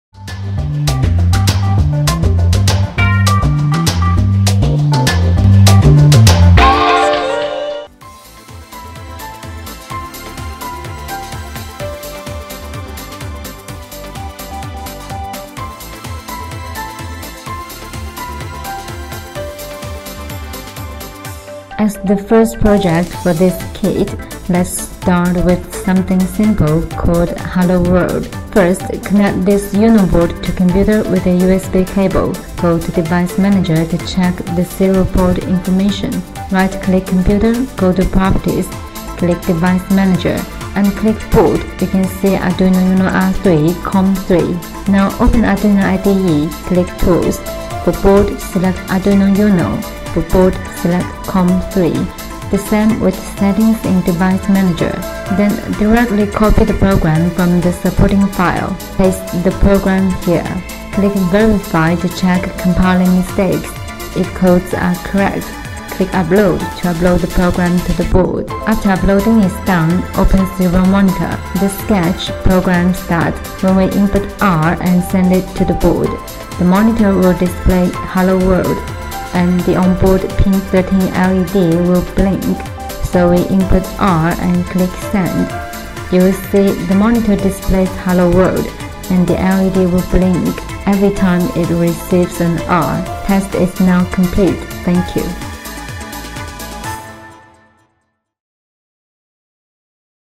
Super kit: Hello World
In this project, you only need an Arduino and a USB cable to start the "Hello World!" experiment.